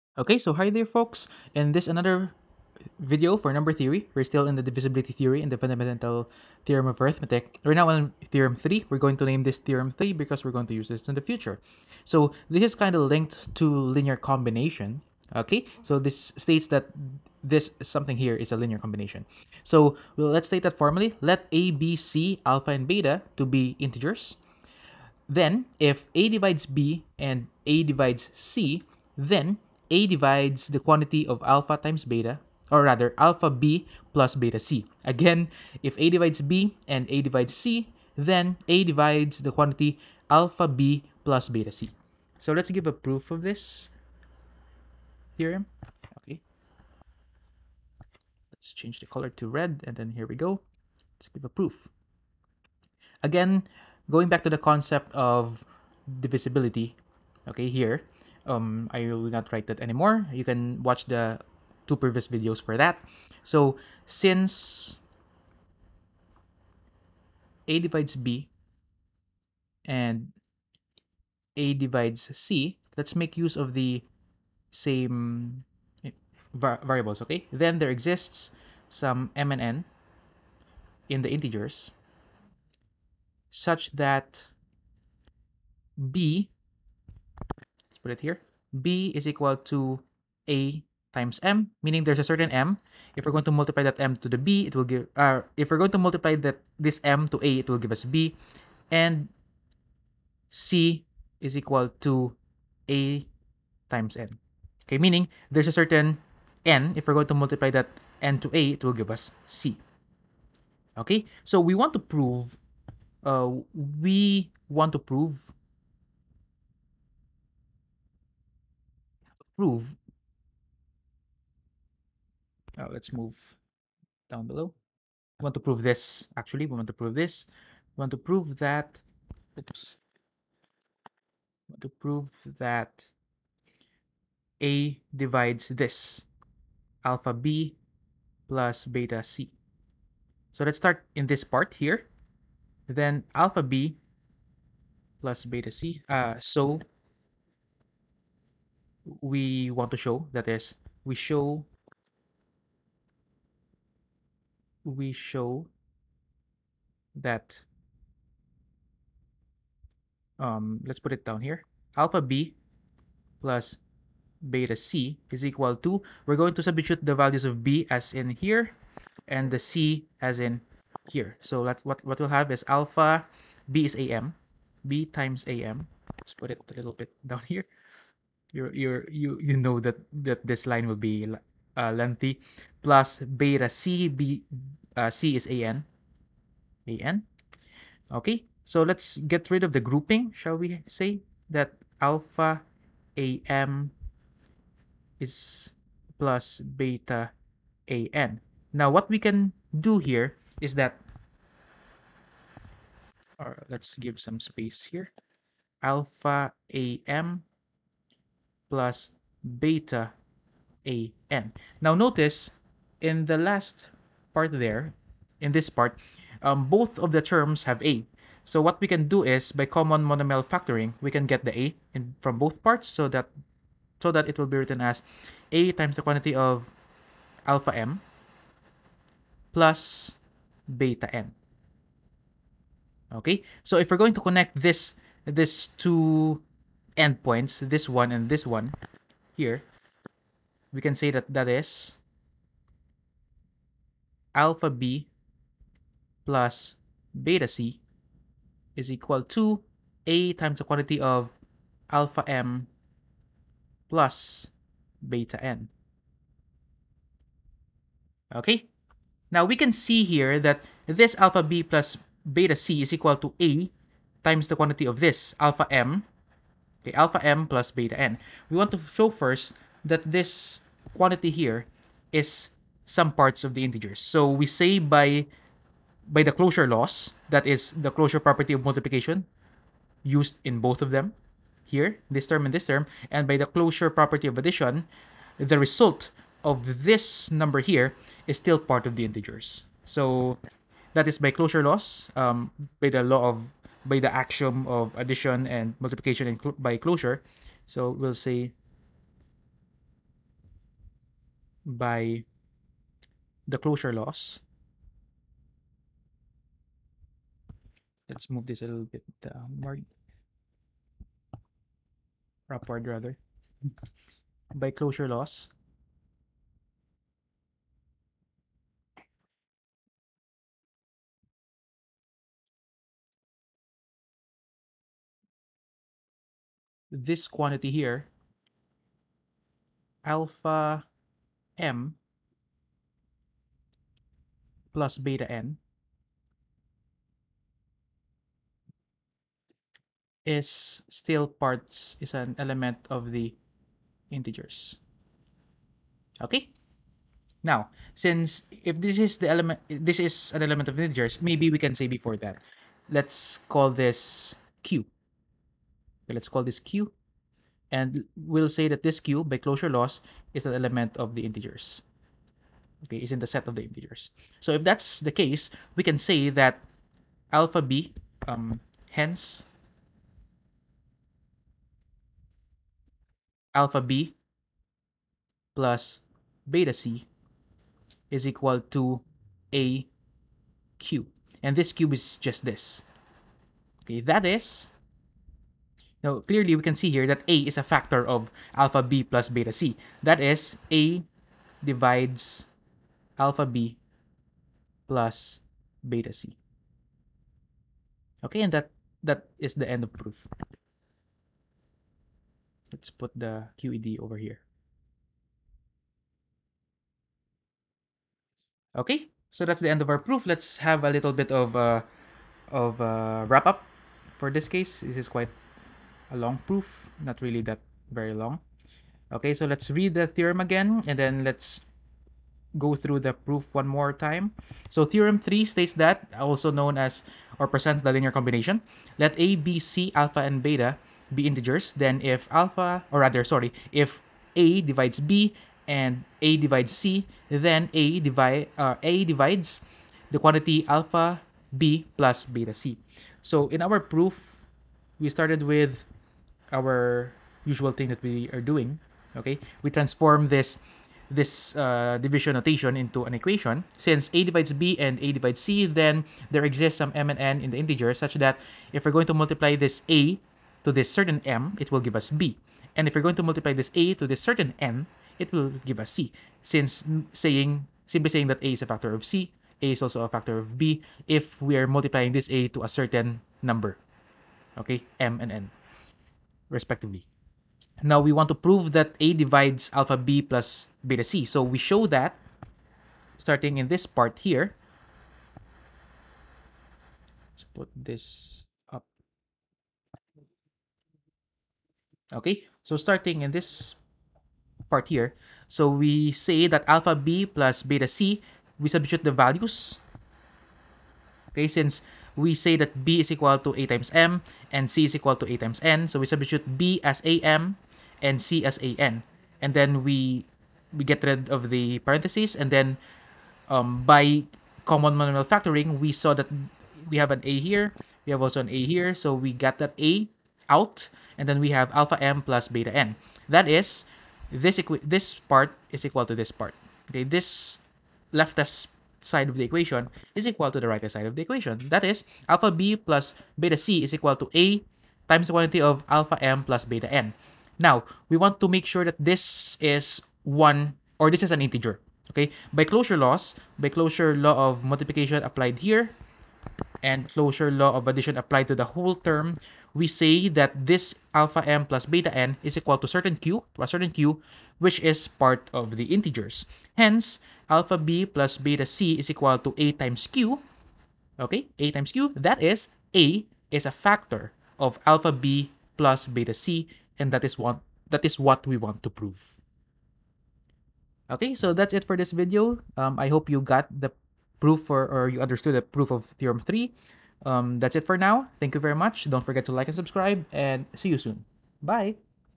Number Theory 20 - Theorem 3: If a|b and a|c, then a|(⍺b+βc)
In this video, we present the Theorem that states that if a|b and a|c, then a|(⍺b+βc) (If a divides b and a divides c, then a divides alpha times b plus beta times c). And we then, write the proof for this Theorem.